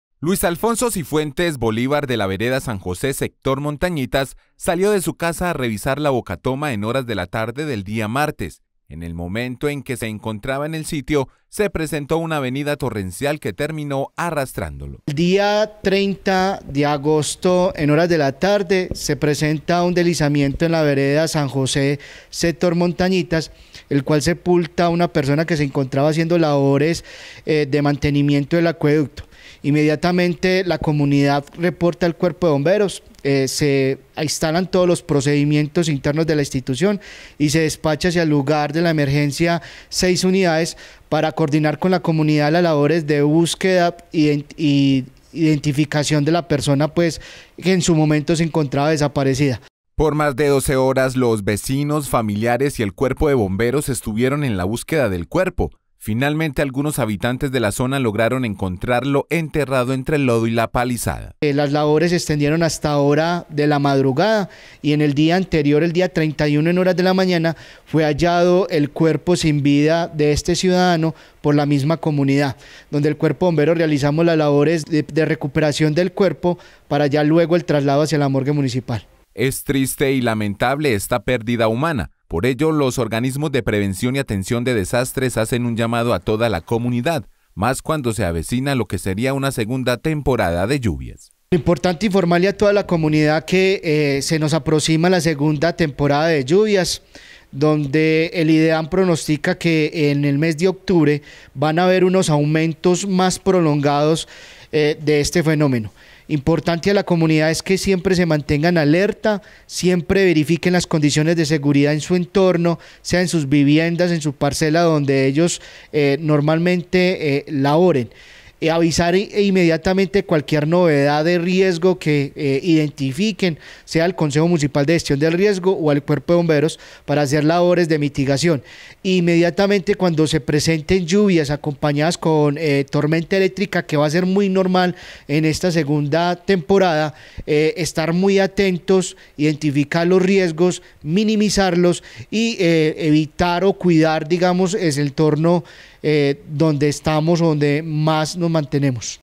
Lamentable, avenida torrencial deja un muerto
Muere un ciudadano en zona rural del municipio, un fenómeno natural que puede estar asociado a las continuas lluvias. El lamentable hecho ocurrió en el sector rural Montañitas